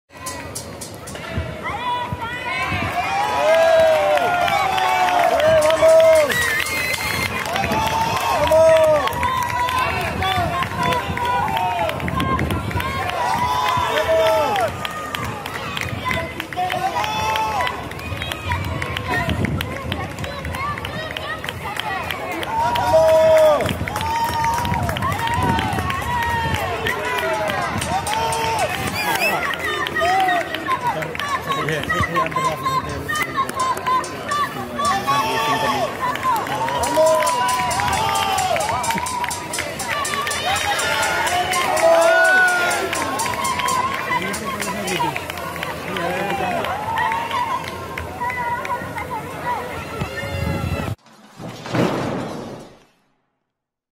Real Zaragoza-Real Oviedo: Así Llegó el Equipo Maño a La Romareda
Llegada del Real Zaragoza para enfrentarse al Real Oviedo en la jornada 23 de LaLiga 1|2|3.